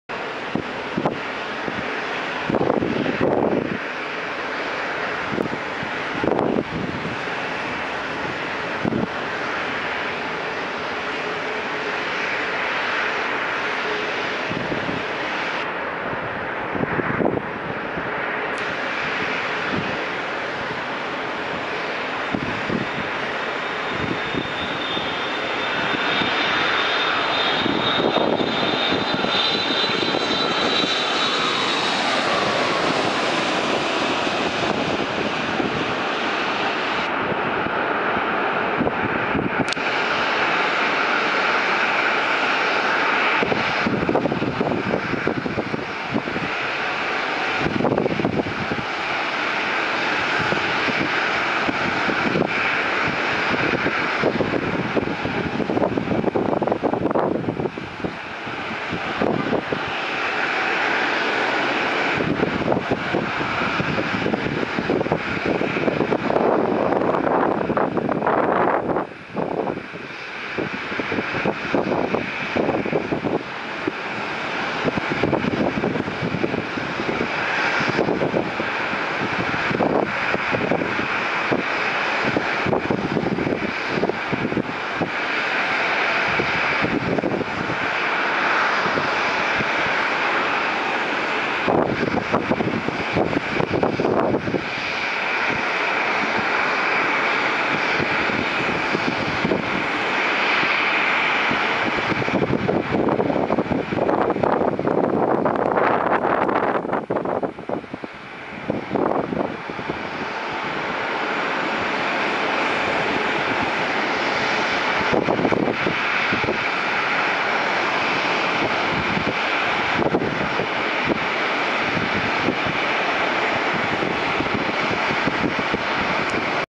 Japan Airlines Boeing 787-8 Dreamliner JA828J SAN Landing 27
Arriving direct from Tokyo-Narita International Airport in Chiba Prefecture. Previously in the Studio Ghibli special livery, which was designed by children in a contest JAL held 4 years ago. JAL is the only airline with direct service from San Diego to Tokyo.
JL66-NRT-SAN